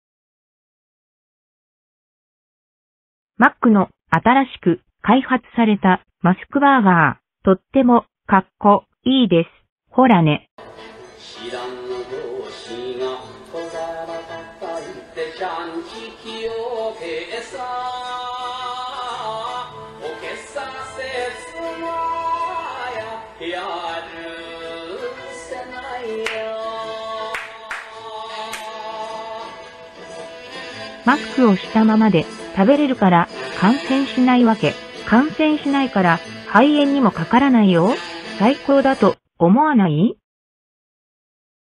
再放送）マックの新発売「コロナ・マスク・バーガー」 マスクのまま食べれる衛生バーガー
アベノマスクの官邸が、共同企画を持ちかけてきたそうだ